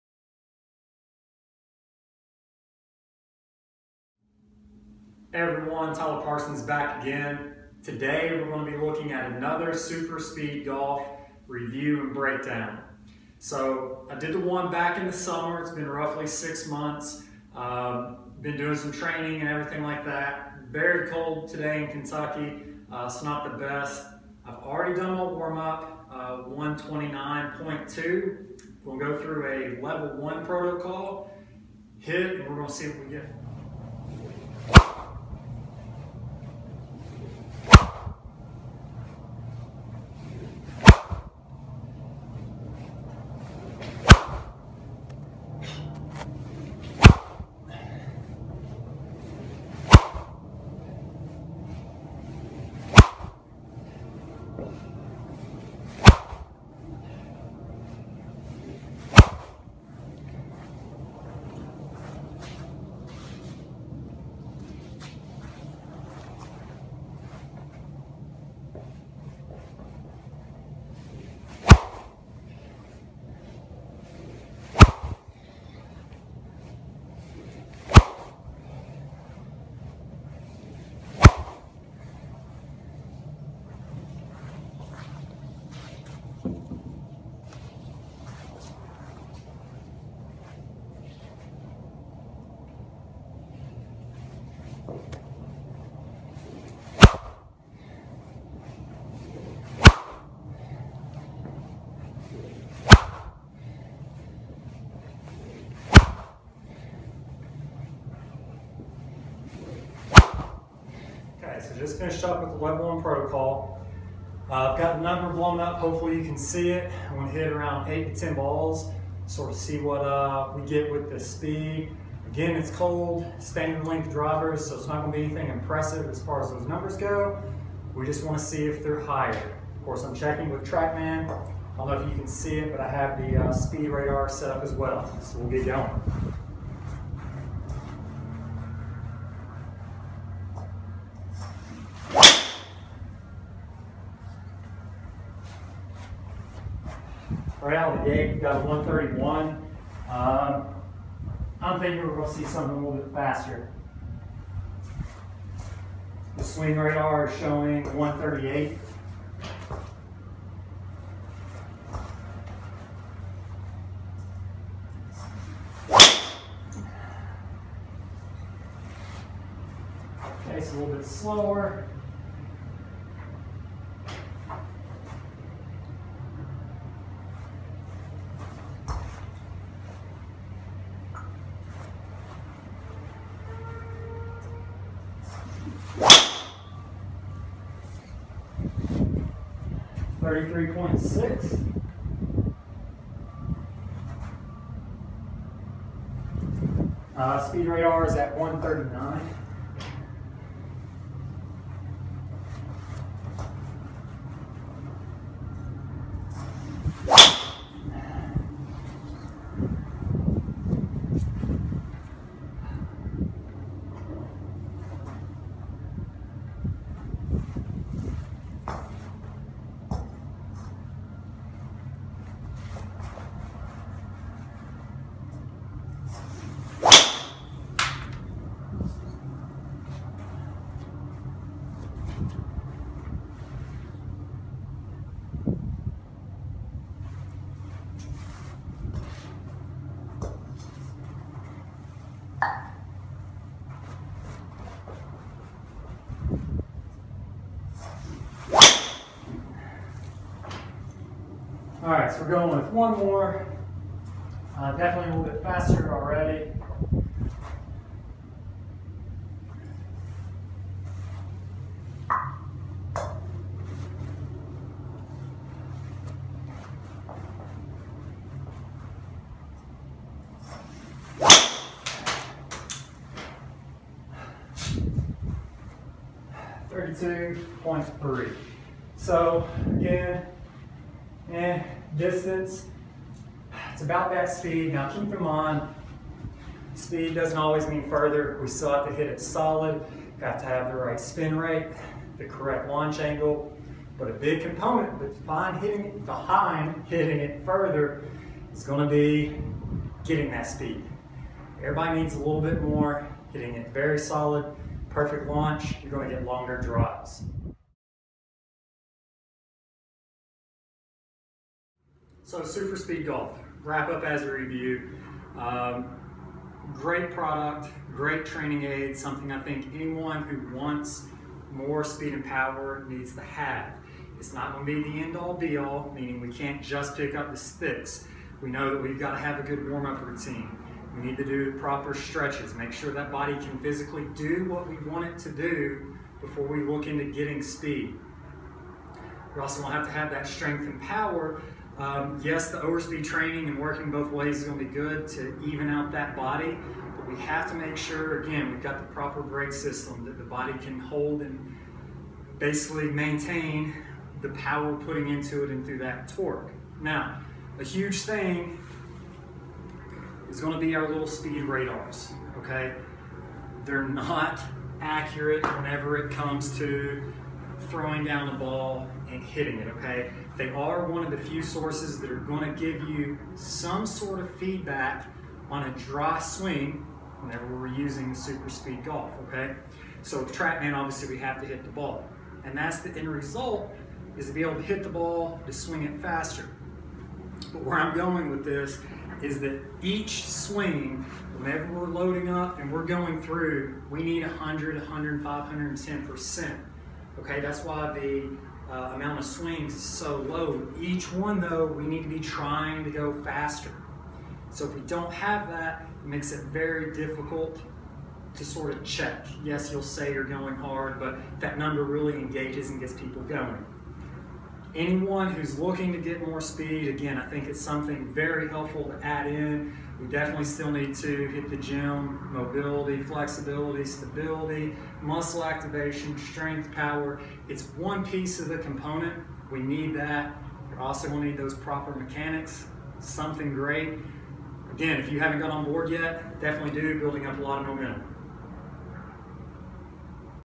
Superspeed golf protocol review BEFORE and AFTER
Here is a review of the superspeed golf protocol as I go through it. I used a before and after for speed. You gain a great insight into the intensity I train and how you should too.